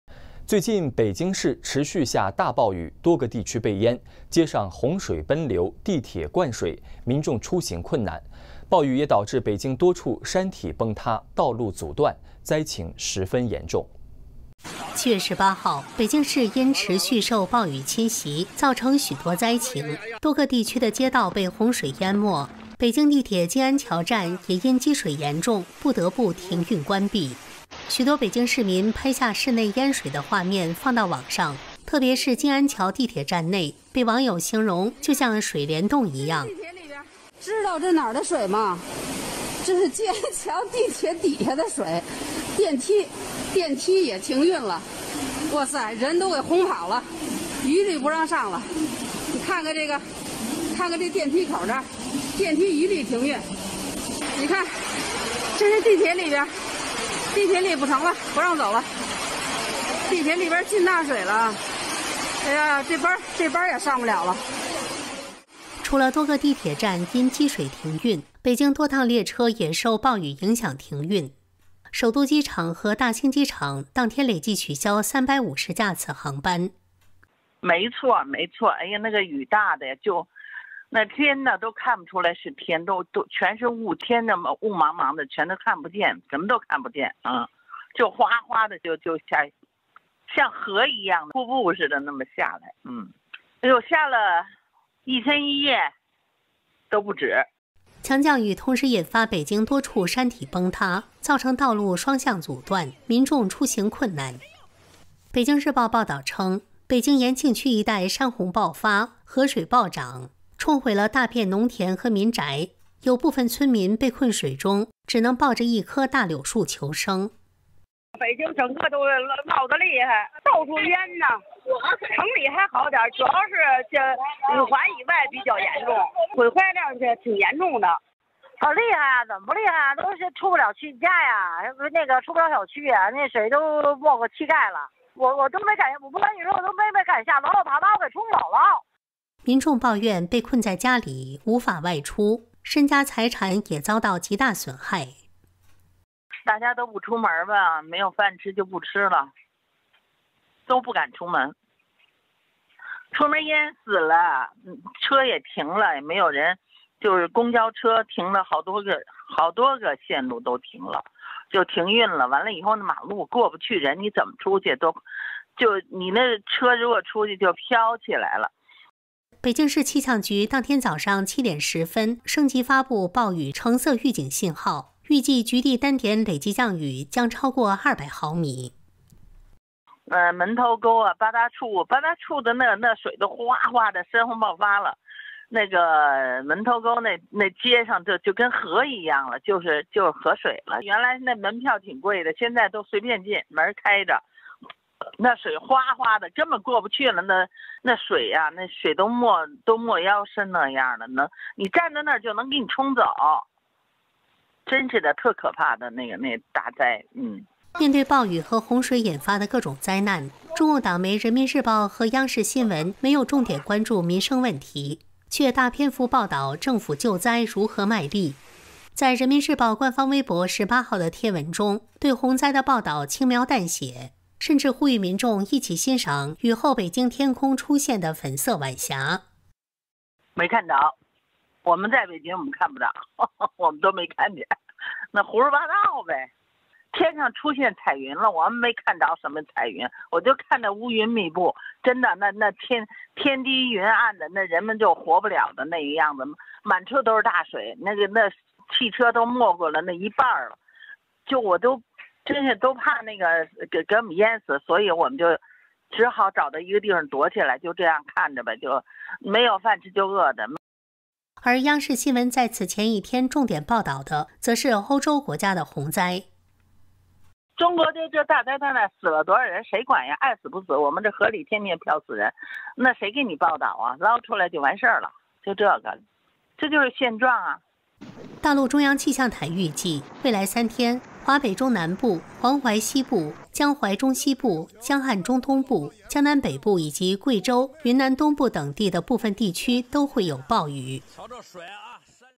【禁聞】北京暴雨 山洪爆發 街道被淹 汽車被沖走 市民不敢出門 人民日報卻稱「好浪漫」
北京市持續下大暴雨，多個地區被淹，街上洪水奔流，地鐵灌水，民眾出行困難，暴雨也導致北京多處山體崩塌，道路阻斷，災情十分嚴重。